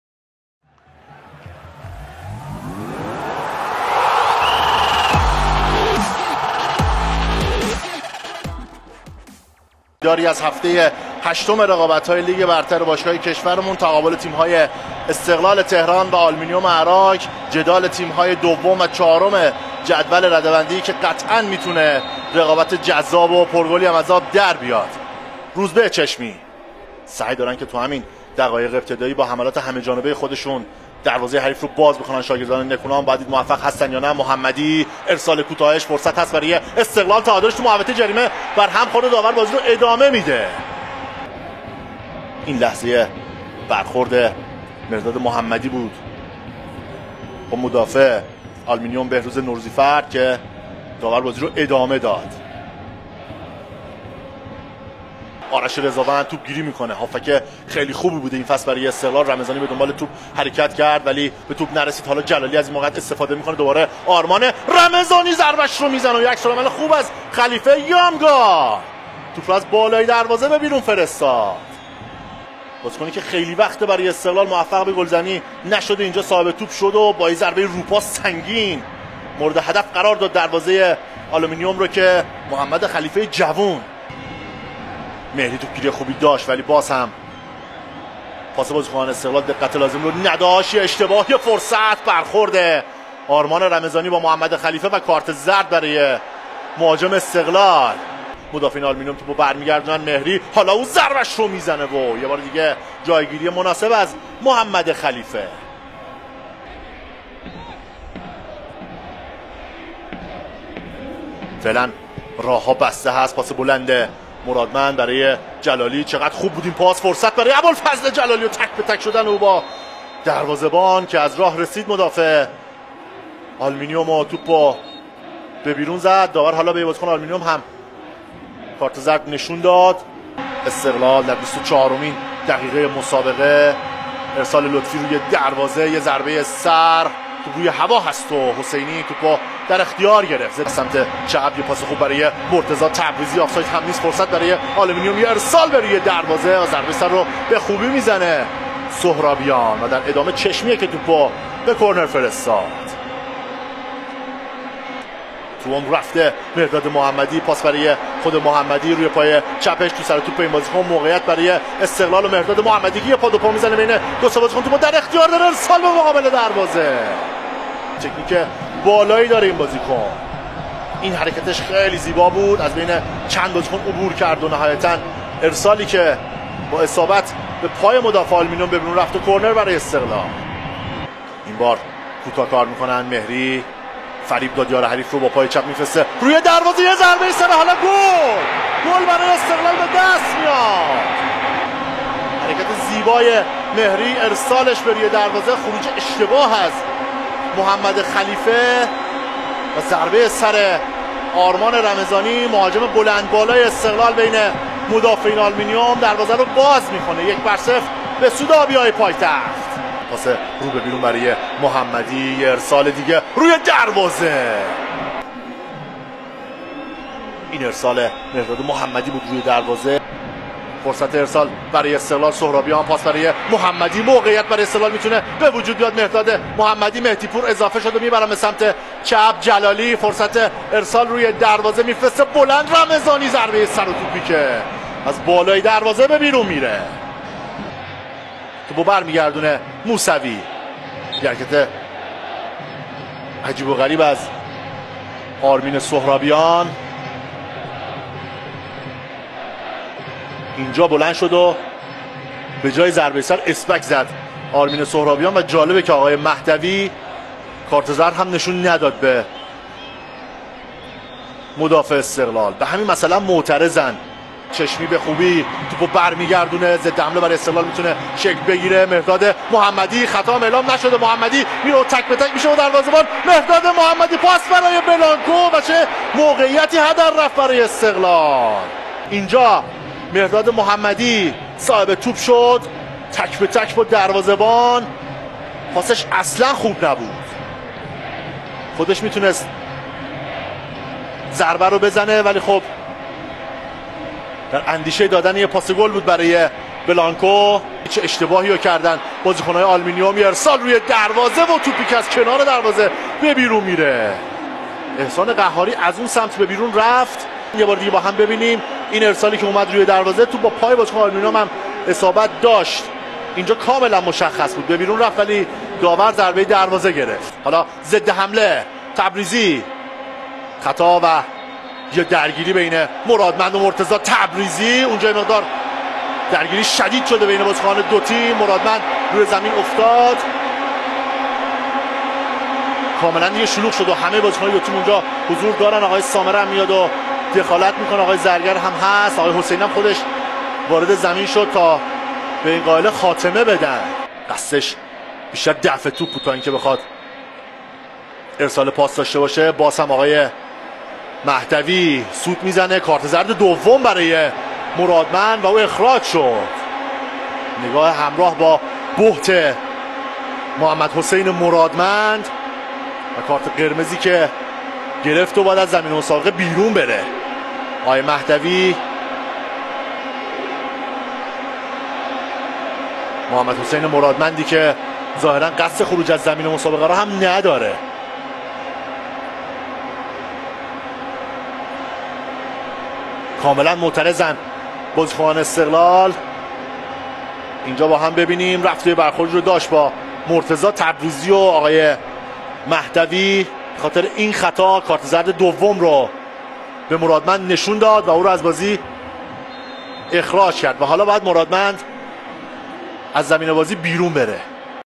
خلاصه بازی جذاب استقلال و آلومینیوم اراک - هفته هشتم لیگ برتر فوتبال ایران
خلاصه استقلال و آلومینیوم اراک - هفته هشتم لیگ برتر فوتبال ایران

شاگردان سید مجتبی حسینی پس از پیروزی روحیه‌بخش در دیدار هفته قبل مقابل نساجی مازندران امشب در ورزشگاه آزادی برابر استقلال قرار می‌گیرند و انگیزه زیادی برای جدال با آبی پوشان دارند.

بازیکنان آلومینیوم اراک که در دیدار قبلی مقابل نساجی مازندران نمایش درخشانی داشتند و در این دیدار خارج از خانه سه امتیاز را کسب کردند، حالا هم با روحیه مضاعف به استقبال دیدار با استقلال آمده‌اند و می‌خواهند بهترین عملکردشان را در استادیوم آزادی به نمایش بگذارند.

باشگاه آلومینیوم اراک هم در آستانه دیدار این تیم مقابل استقلال تصویری از حلقه اتحاد بازیکنان‌شان در وسط ورزشگاه آزادی منتشر کرده که در نوع خود جالب توجه است.